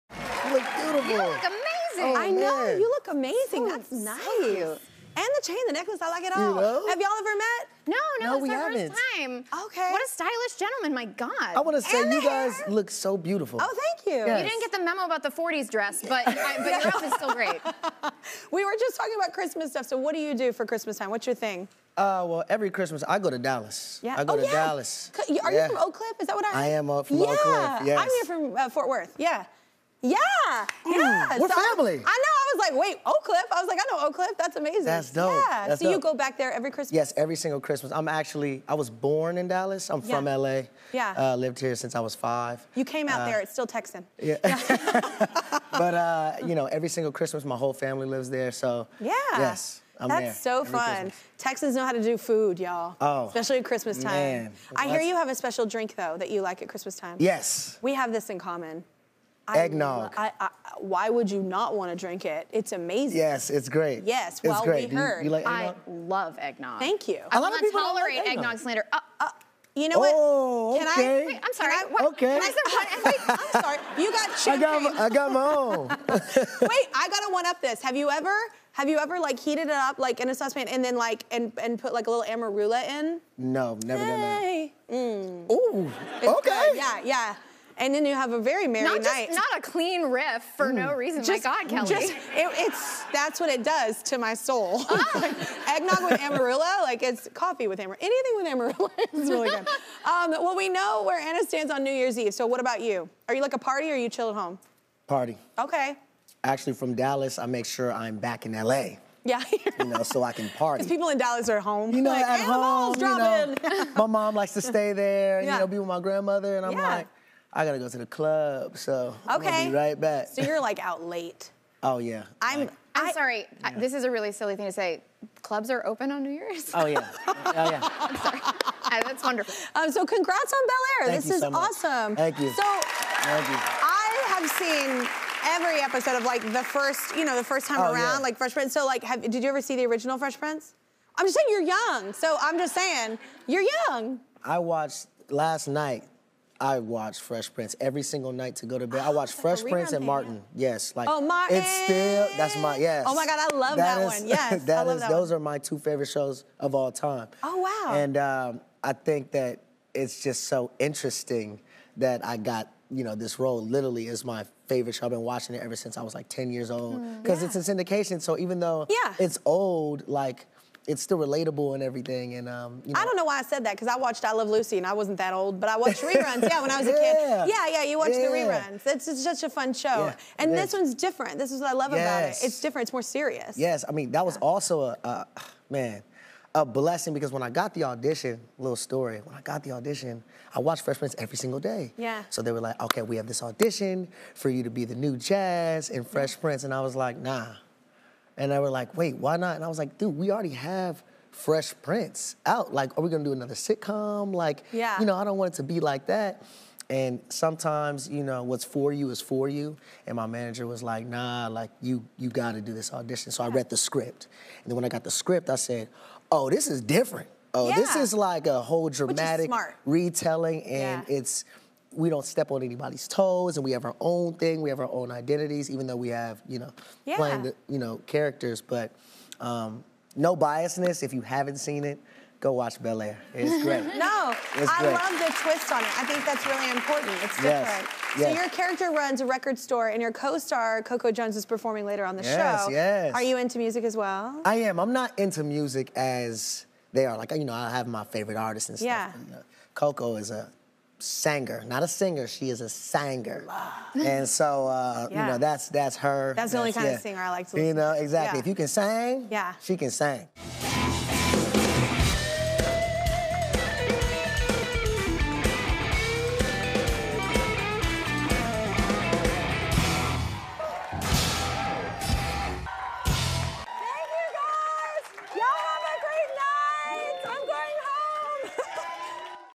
Jordan L. Jones & Kelly Clarkson Bond Over Christmas In Texas: 'We're Family!'
Jordan L. Jones dishes on the "blessing" of landing his role as Jazz in the Peacock series "Bel-Air," and admits he watched the original "Fresh Prince of Bel-Air" series every night before going to bed growing up. Jordan also bonds with Kelly over going to Texas for Christmas, and shares his plans for New Year's Eve.